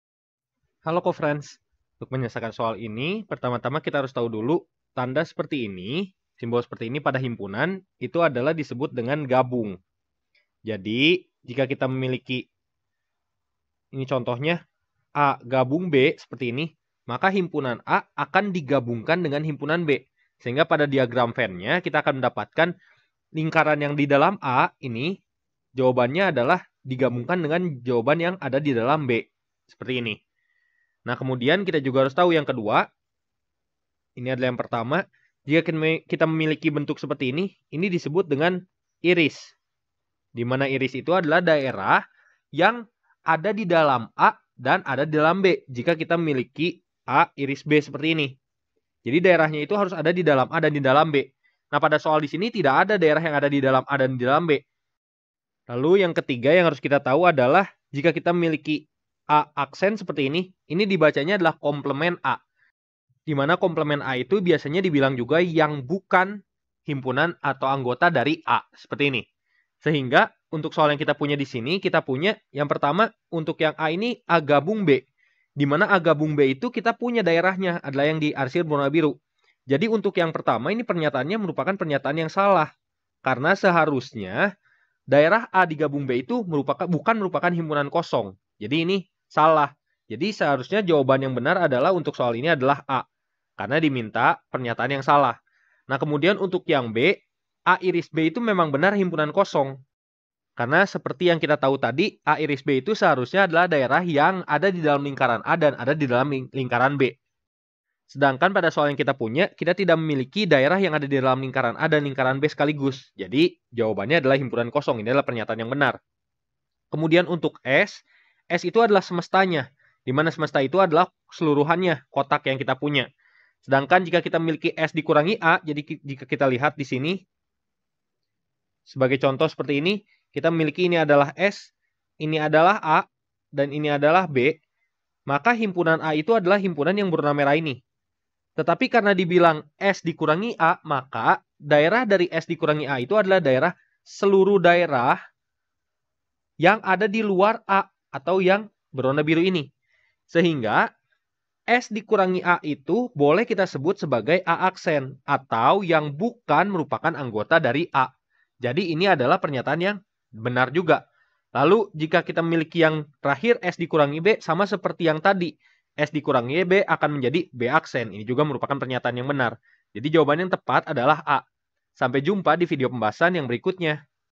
Pernyataan manakah yang salah sesuai dengan S A B gambar di bawah? a. A U B = C b. Ao B = / CS-A ...
Pernyataan manakah yang salah sesuai dengan S A B gambar di bawah? a. A U B = C b. Ao B = / CS-A = A' d.S-B = B'

Topik atau Materi: Penggunaan Diagram Venn untuk Irisan dan Gabungan Himpunan - HIMPUNAN - ALJABAR - Matematika Kelas 7 SMP - KTSP

Sumber Soal: Matematika Kelas 7 Edisi 4, KTSP, AW, 2008 , Depdiknas

00:00 : Bedah Soal
01:00 : Konsep, rumus dan pengertian Penggunaan Diagram Venn untuk Irisan dan Gabungan Himpunan
01:18 : Langkah penyelesaian soal
01:54 : Jawaban akhir
02:24 : Penutup